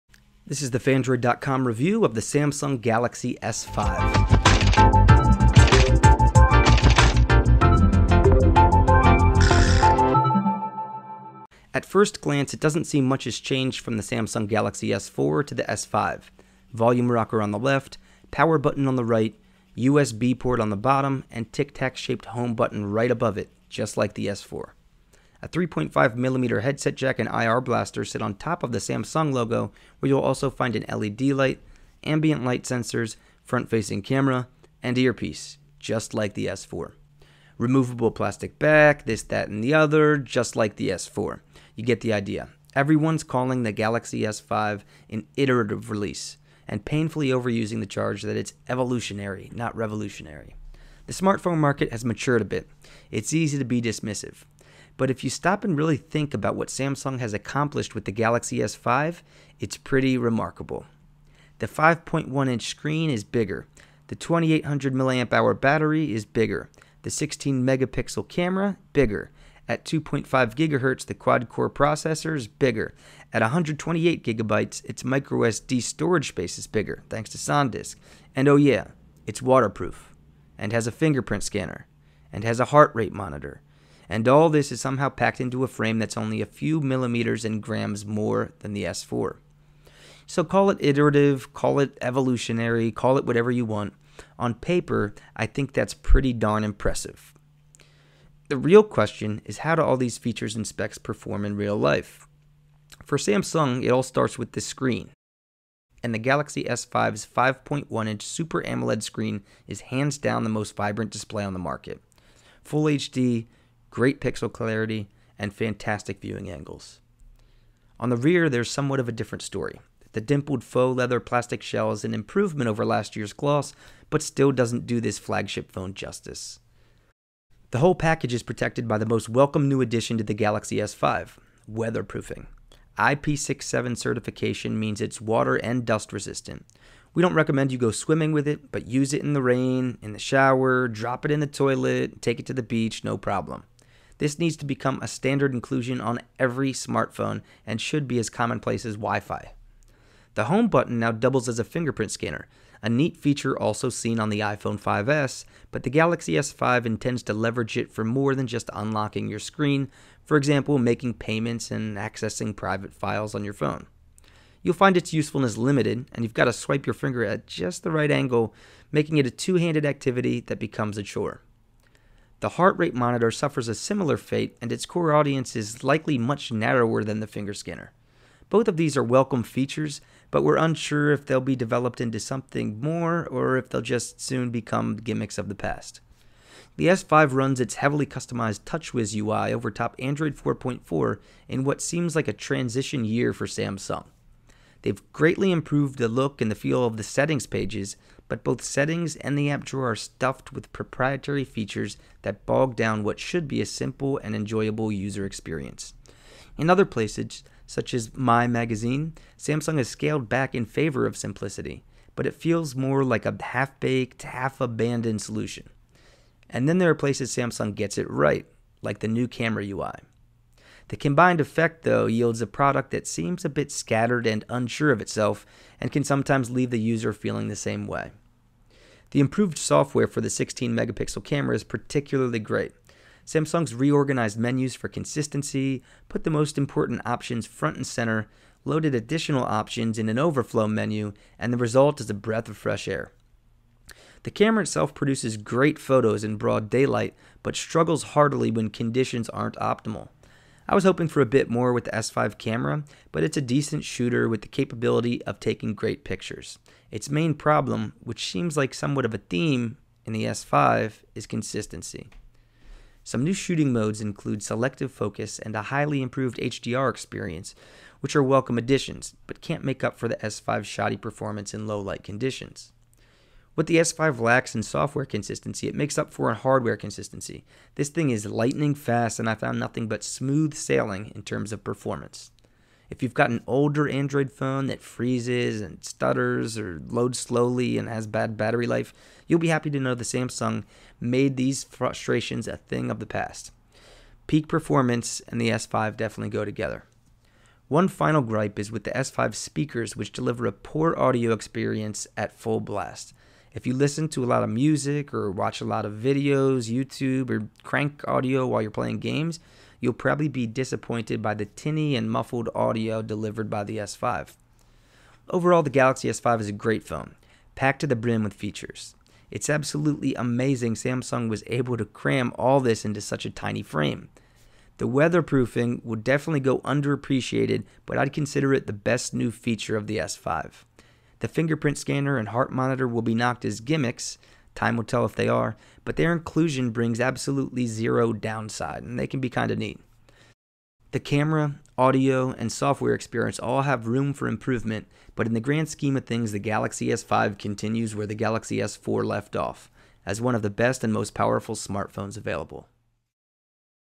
Samsung Galaxy S5 Review
Samsung's latest flagship, the Galaxy S5, has been called an iterative update to last year's Galaxy S4, but that doesn't tell the whole story. Find out why in our full video review for more.